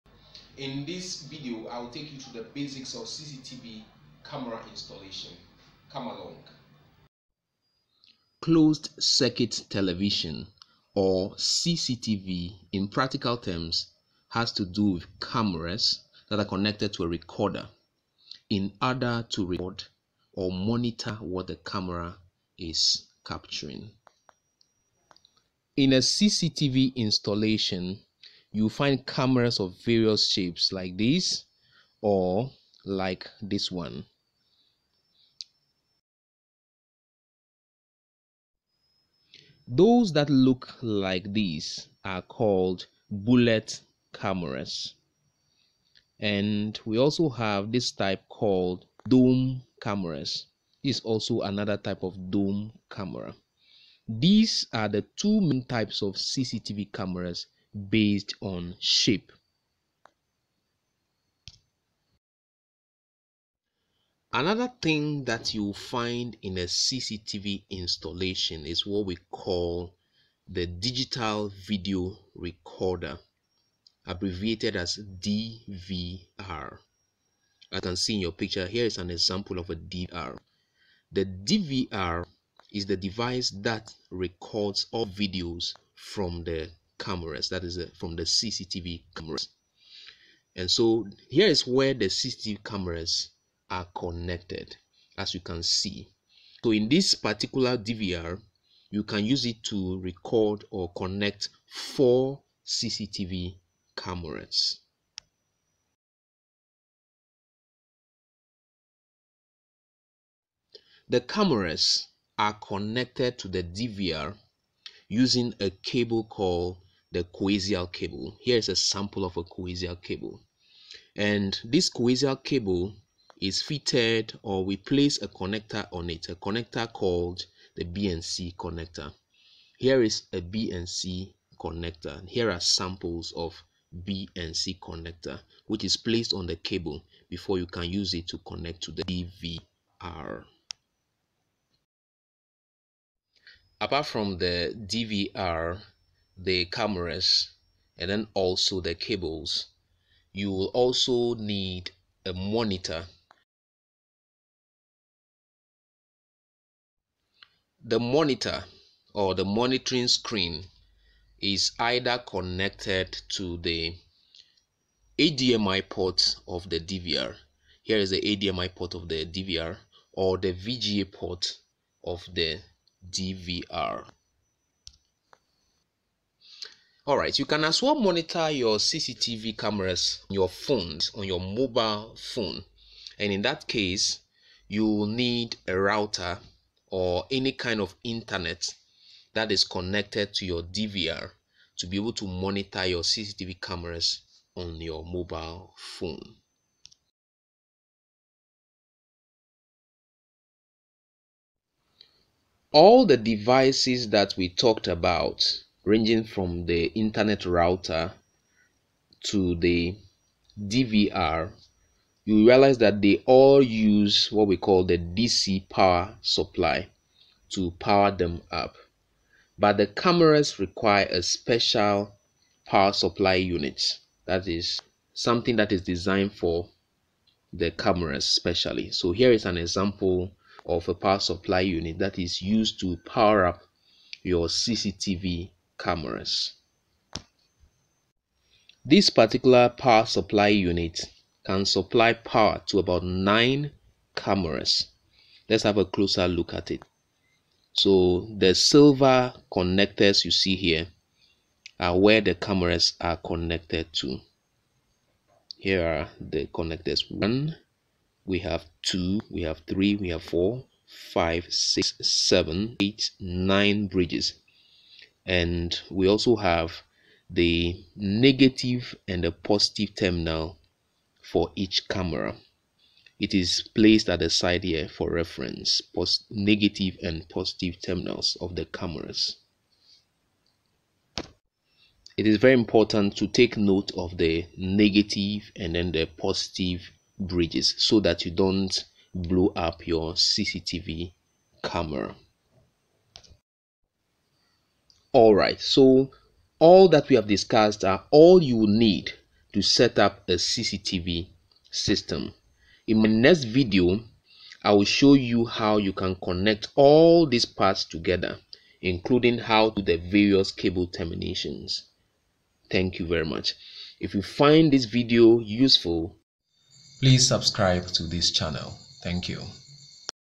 CCTV Camera Installation (Beginners guide)
CCTV camera Installation, Basic Lesson for Beginners,
Components that are put together to setup a CCTV system,
CCTV Cameras,
Digital Video Recorders,(DVR)
Coaxial cable,
BNC Connector,
CCTV Power Supply Unit.